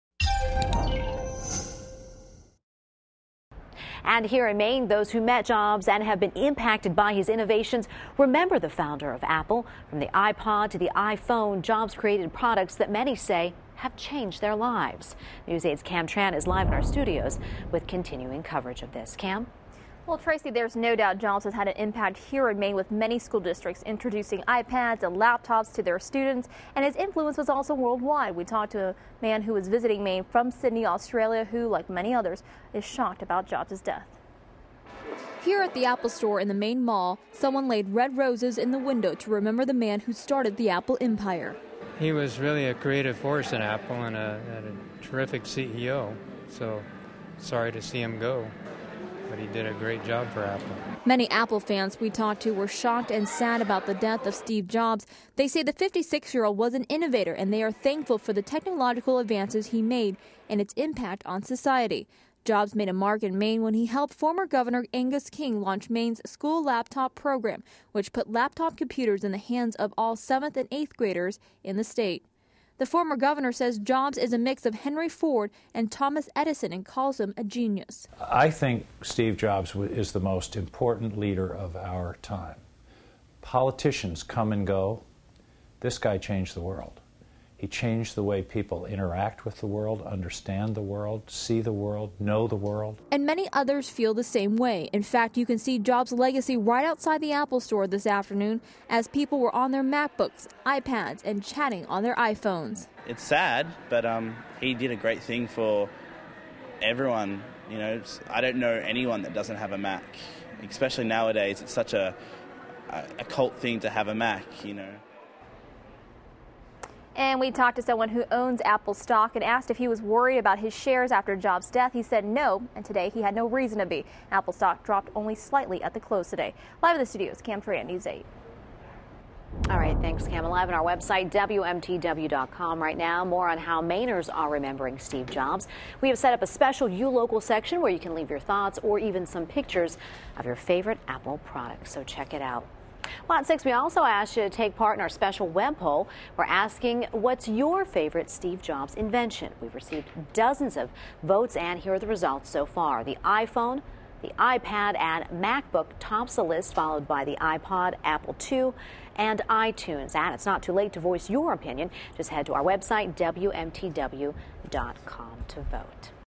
People In Maine Remember Steve Jobs
From visitors to former governor Angus King, people across Maine are remembering Steve Jobs.  News 8's Cam Tran reports.